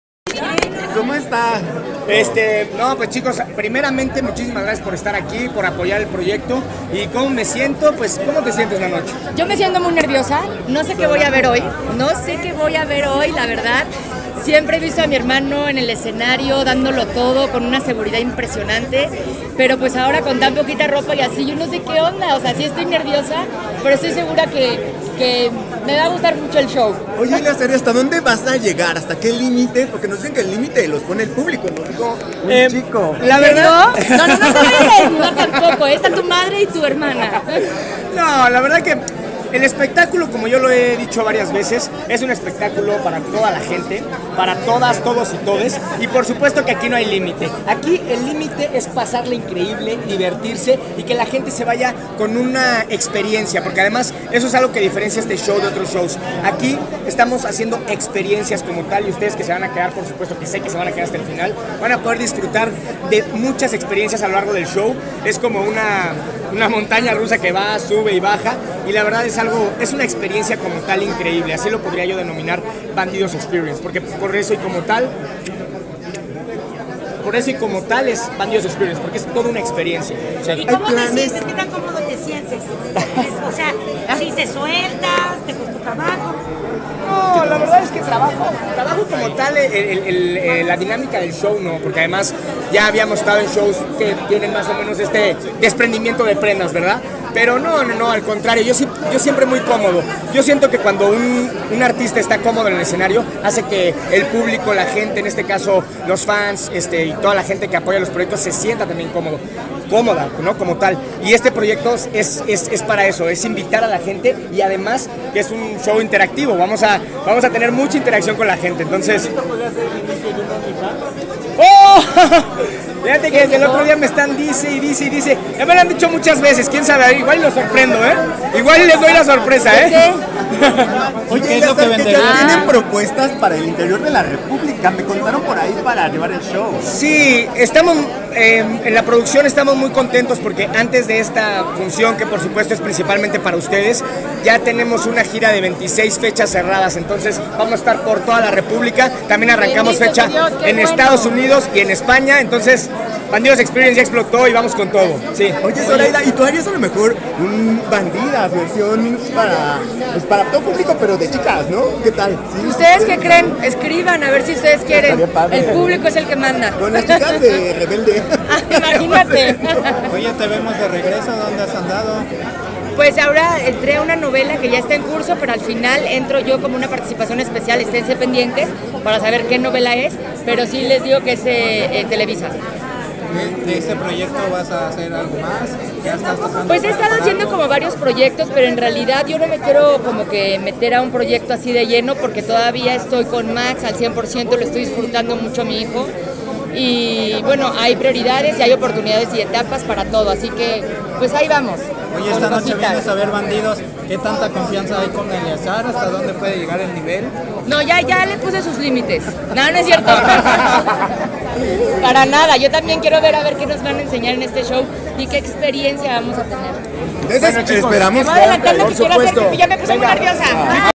Eleazar Gómez show, 🔥Bandidos🔥 para ellos, ellas, elles.
Eleazar Gómez es el protagonista del show cand3nt3 "BANDIDOS Experience" y nos platica hasta dónde llegará con sus bailes.